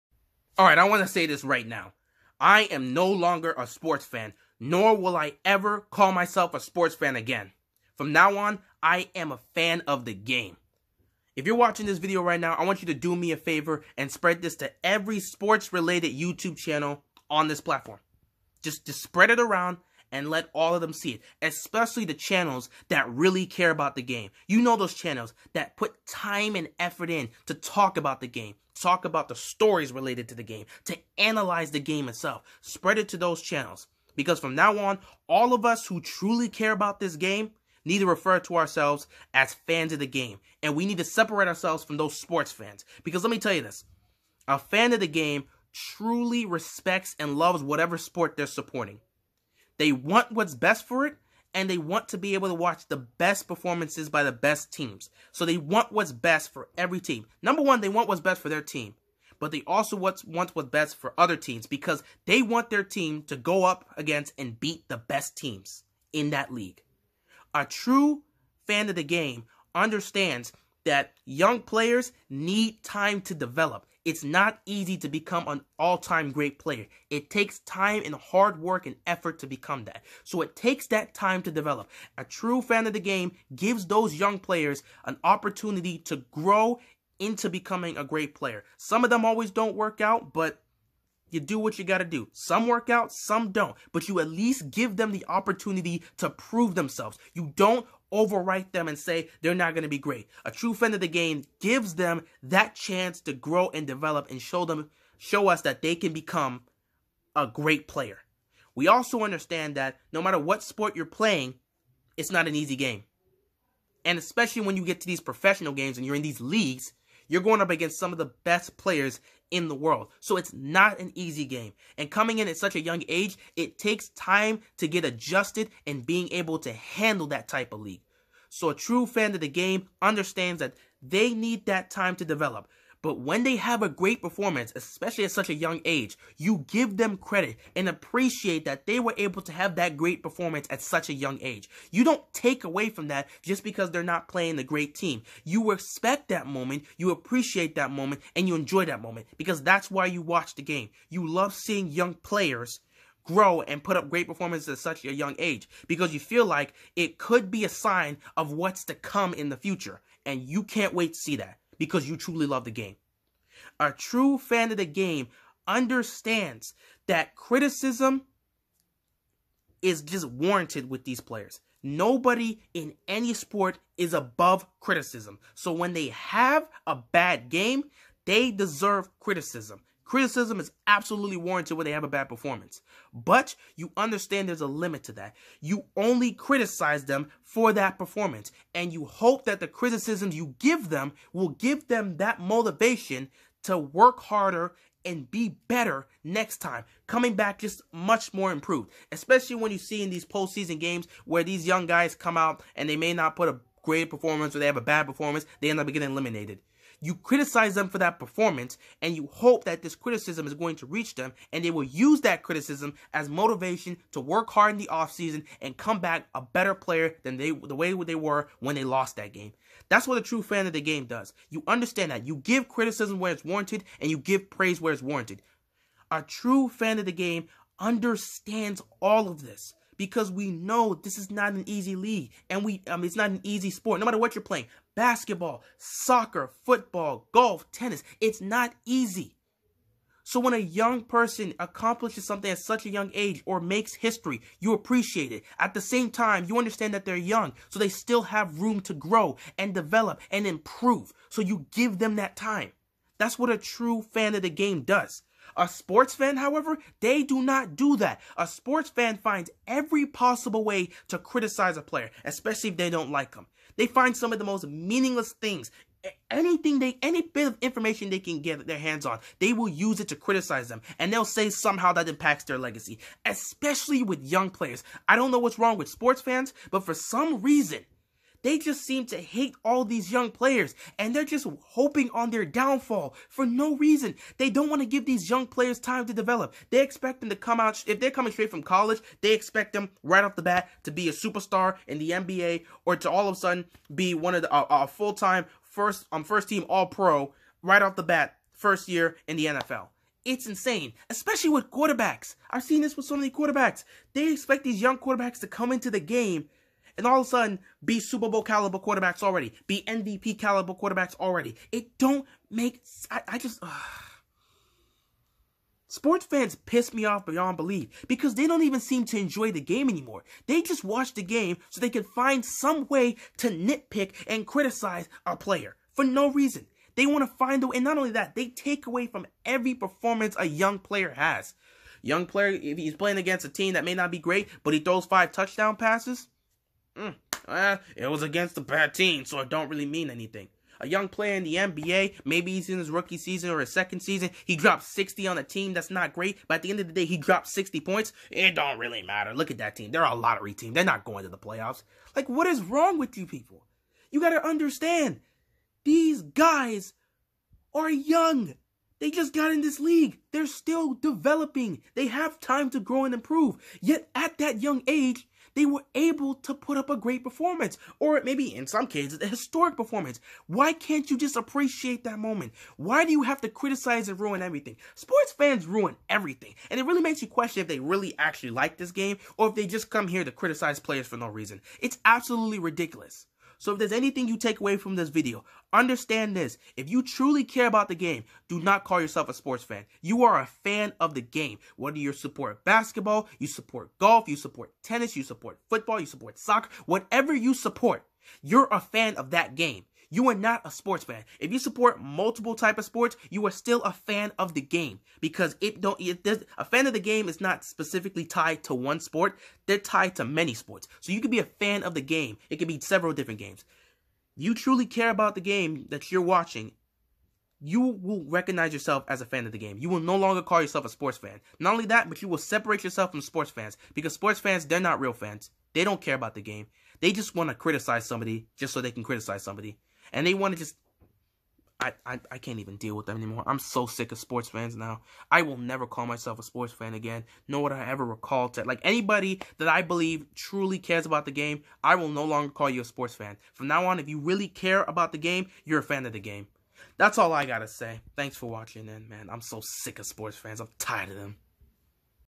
I WILL NO LONGER CALL MYSELF A SPORTS FAN
I am done with sports fans. they don't seem to appreciate the game. the just spend their time criticizing every player that's not on their team. I am no longer a sports fan. I am a fan of the game.